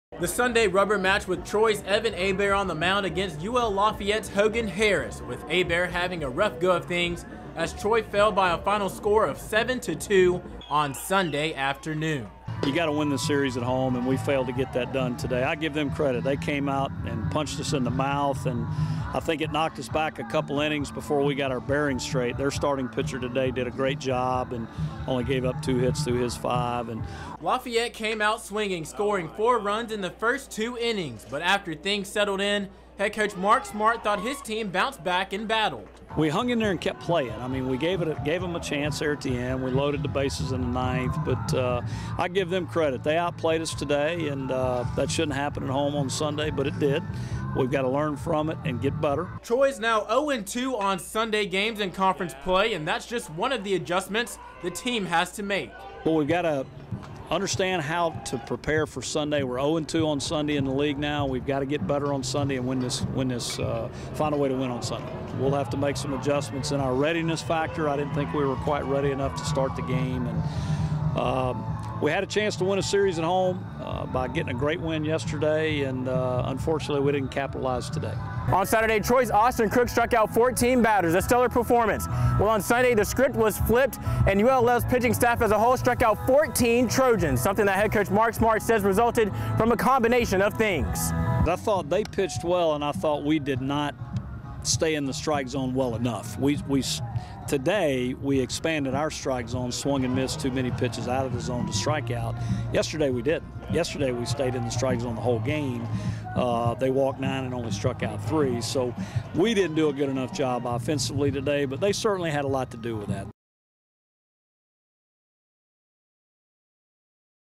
TROY Baseball vs Louisiana Lafayette - TROY TrojanVision News
The TROY baseball team dropped the final game of their three game series against Louisiana Lafayette. TROY TrojanVision News Jahmal Kennedy has the highlights.